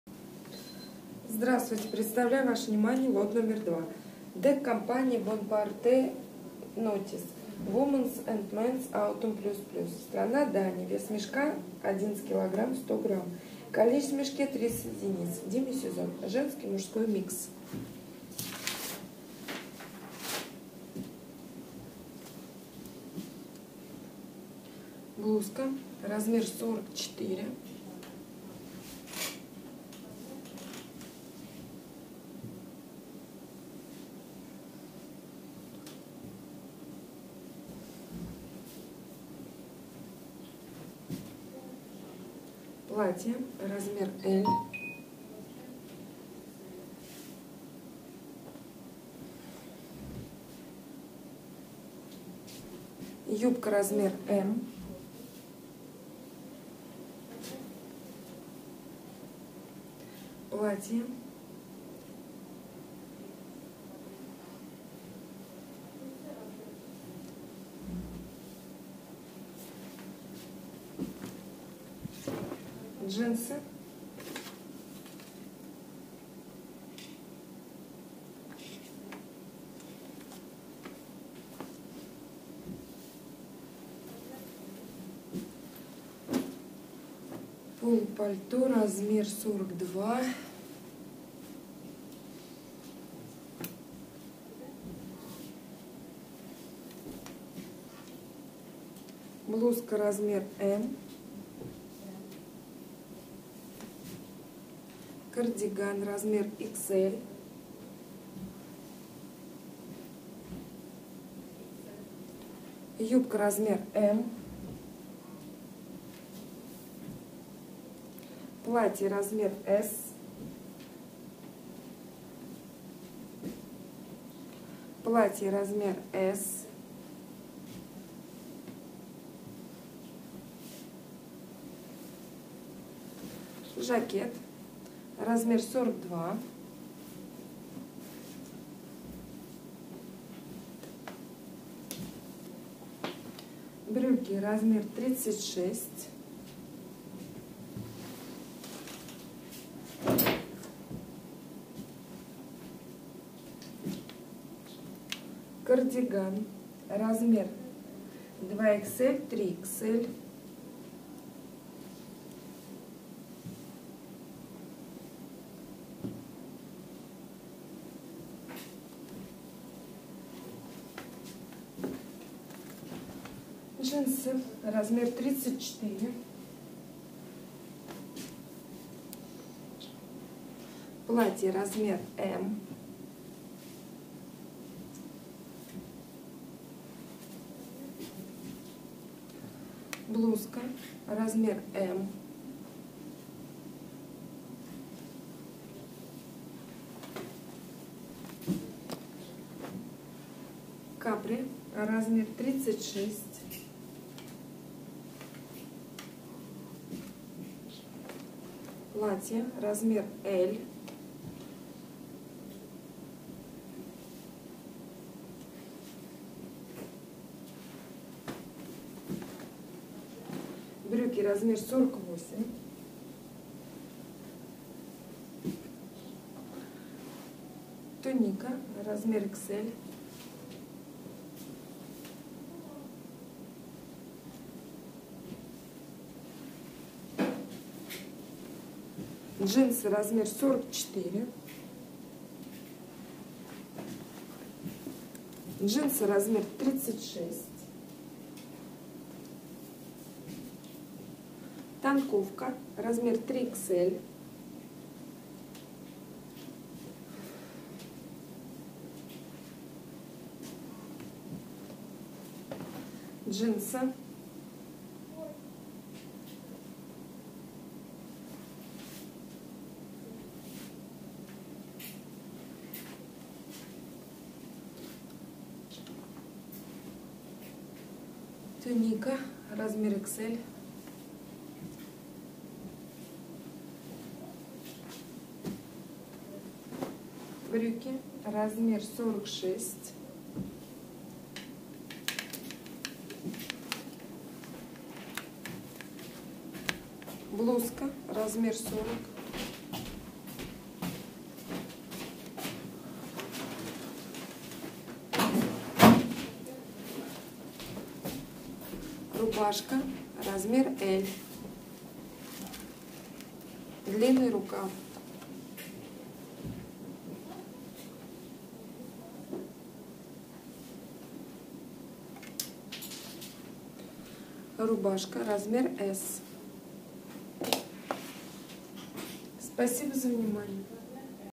DK Company Bona`Parte, Notice Womens&mens autumn 2,сток одежда оптом
DK Company Bona`Parte, Notice Womens&mens autumn (лот №2)
Женский и мужской микс (сток)
Страна: Дания
Сезон: Осень
Цена за кг.: 18.09 евро
Цена за мешок : 200.8 евро